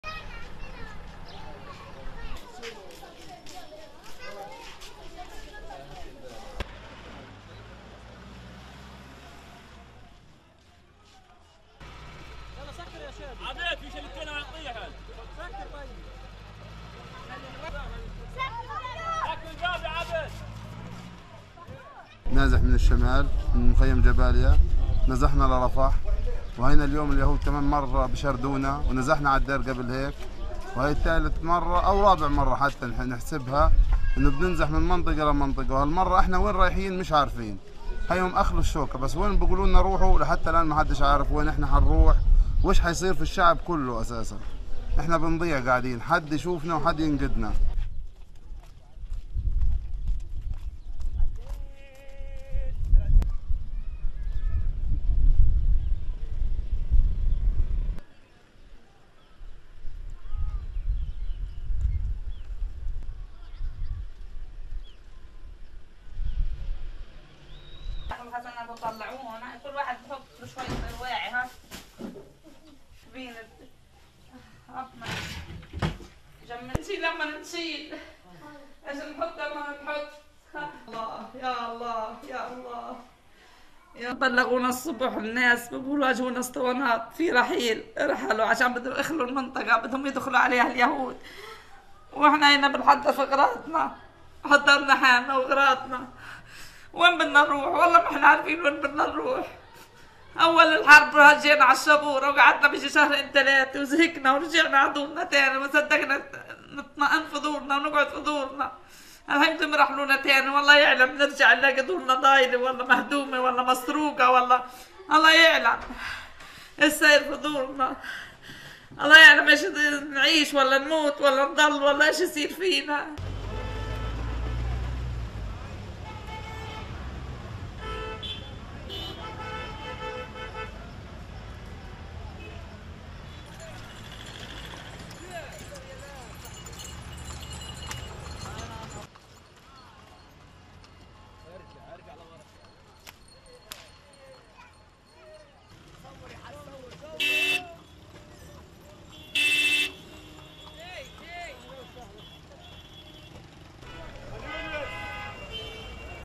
"لا نعرف إلى أين سنذهب هذه المرة".. الجيش الإسرائيلي يدعو إلى إخلاء فوري من رفح…
دعا الجيش الإسرائيلي نحو 100 ألف فلسطيني، الاثنين، إلى البدء في "إخلاء فوري" لمدينة رفح جنوب قطاع غزة، مما يشير إلى أن الغزو البري الموعود به منذ فترة طويلة قد يكون وشيكاً ويزيد من تعقيد الجهود المبذولة للتوسط في وقف إطلاق النار.